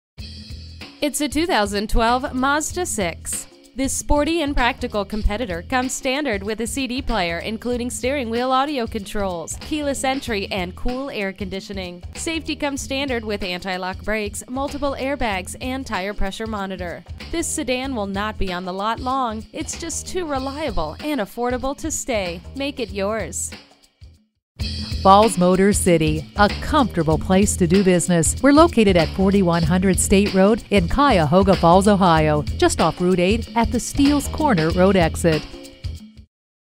2012 Mazda Mazda6 Cuyahoga Falls, OH
Year: 2012
Make: Mazda
Model: Mazda6
Trim: 4dr Sedan Automatic i Touring
Engine: 2.5L 4 CYLINDER
Transmission: 5-Speed Automatic
Color: Steel Blue Mica
Interior: Black
Mileage: 69263
Stock #: J16630A
VIN: 1YVHZ8DH5C5M19082
- This 2012 Mazda Mazda6 4dr 4dr Sedan Automatic i Touring features a 2.5L 4 CYLINDER 4cyl Gasoline engine. It is equipped with a 5 Speed Automatic transmission. The vehicle is Steel Blue Mica with a Black Cloth interior. It is covered by a limited warranty. -

Address:
4100 State Rd